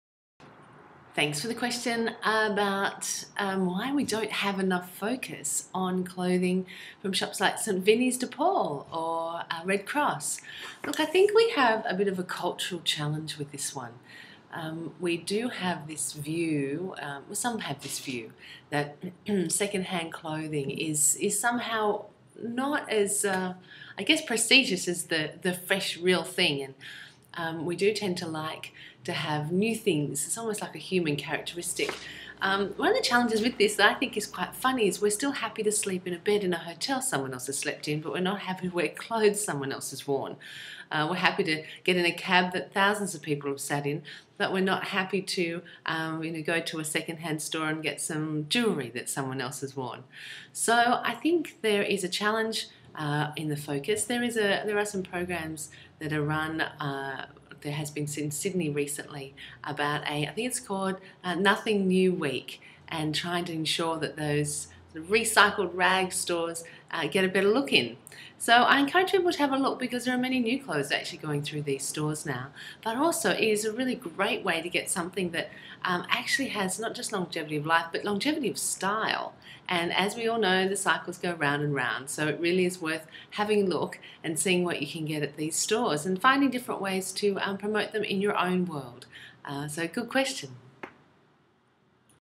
Wearing 2nd hand clothing from charity stores? Bronwyn Darlington WEAR Q & A Series
Bronwyn Darlington our sustainable fashion expert responds to the following question

Why isn't there enough emphasis placed on wearing clothing from shops like St Vincent De Paul shops or the Salvation Army Family Stores?

Do you have a sustainable fashion question you would like answered?